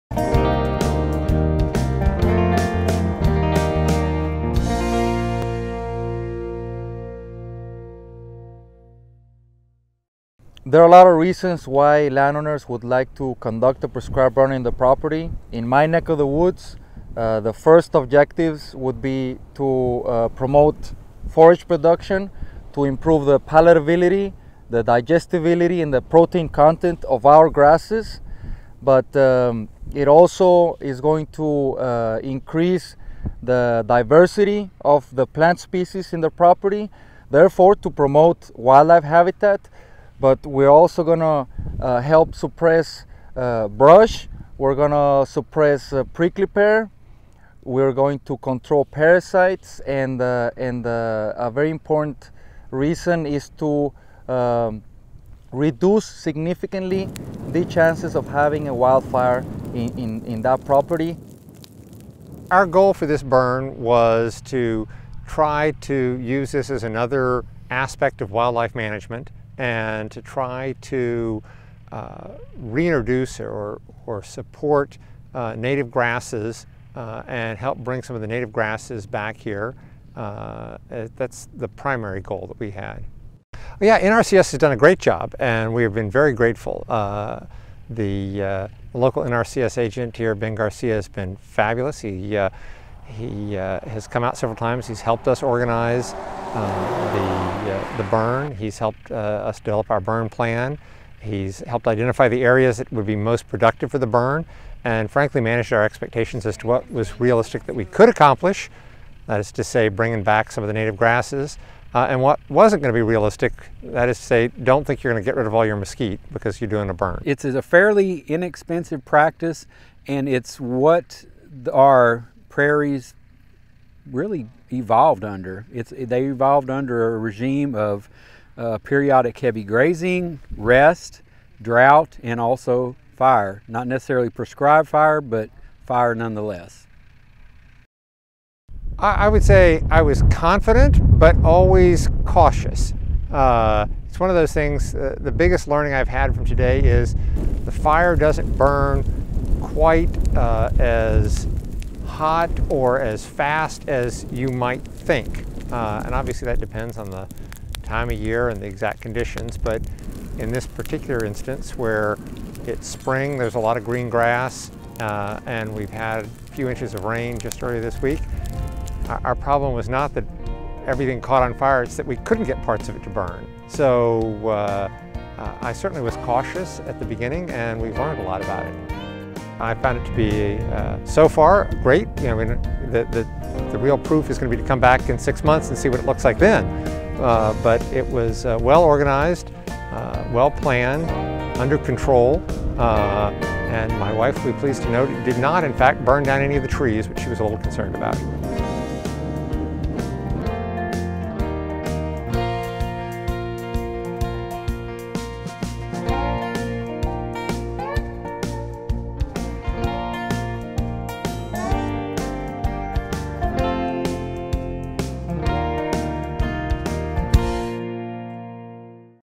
Kerr Creek Farm Benefits from Prescribed Fire
This video describes the benefits to Bob Casey, owner of Kerr Creek Farm near Burton, Texas.  NRCS conducted a prescribed bur n to improve his native grass and enhance wildlife habitat on his farm.